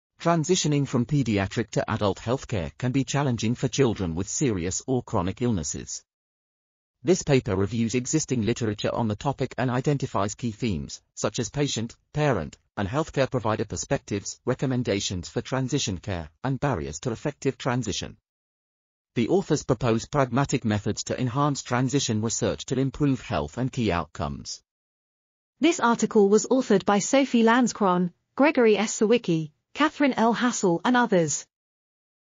Transition to adulthood and adult health care for patients with sickle cell disease o... | RTCL.TV
### Keywords ###

### Article Attribution ###
Title: Transition to adulthood and adult health care for patients with sickle cell disease or cystic fibrosis: Current practices and research priorities
Authors: Sophie Lanzkron, Gregory S. Sawicki, Kathryn L. Hassell, Michael W. Konstan, Robert I. Liem ,and Susanna A. McColley

### Video Timestamps ###
0:00:00 - Summary
0:00:25 - Title
0:00:32 - End